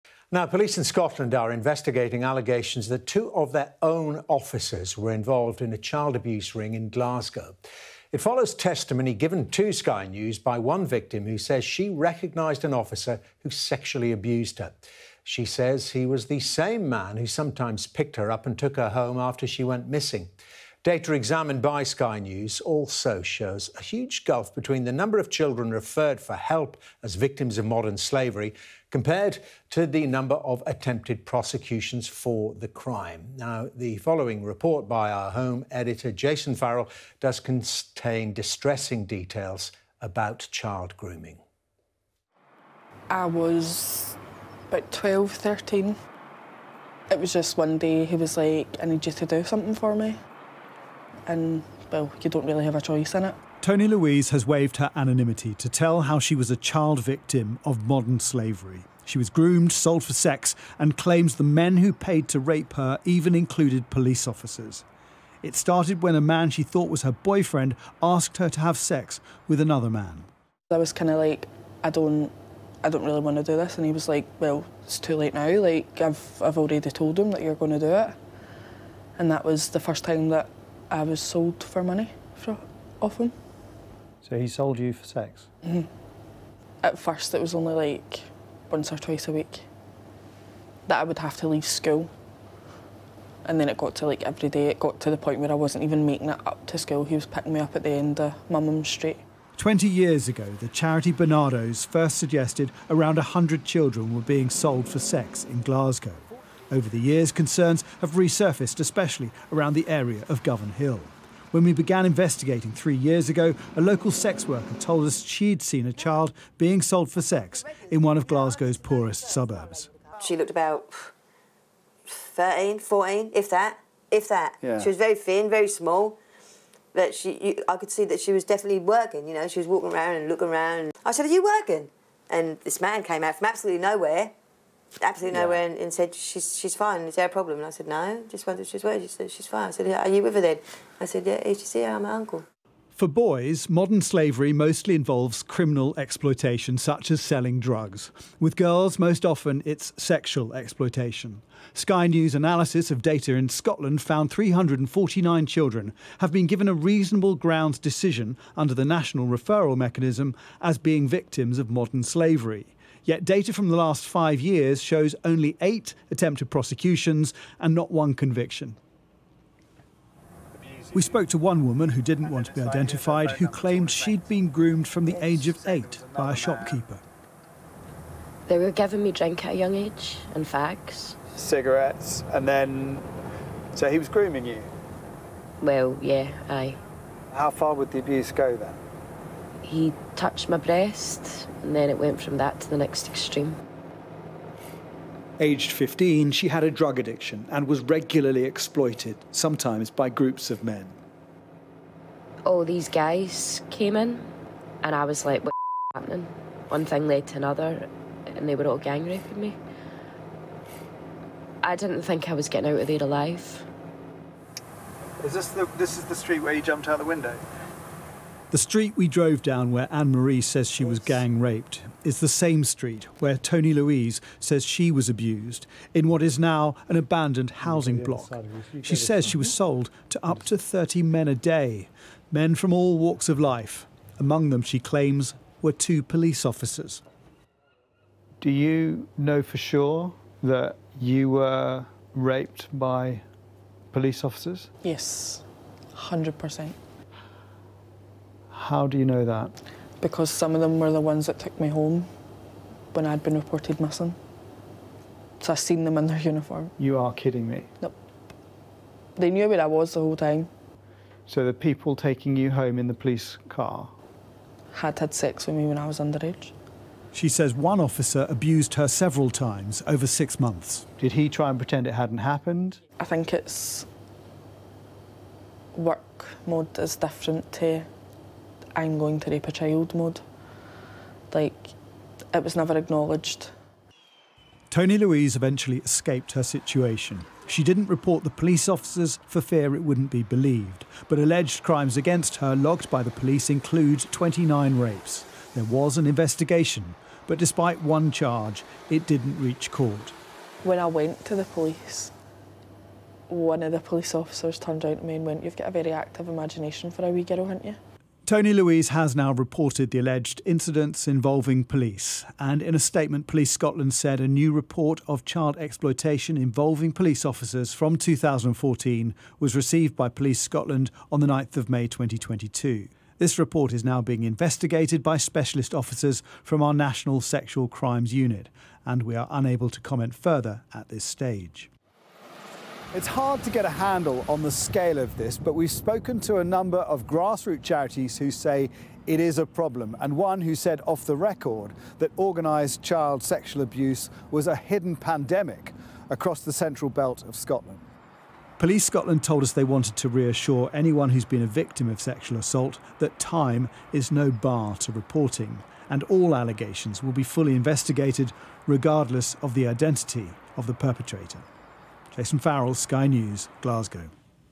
Claims police involved in child abuse in Glasgow
A Sky News investigation has discovered that police in Scotland are investigating allegations that two of their own officers were involved in a child abuse ring in Glasgow.

Warning: this report from Jason Farrell contains distressing details about child grooming.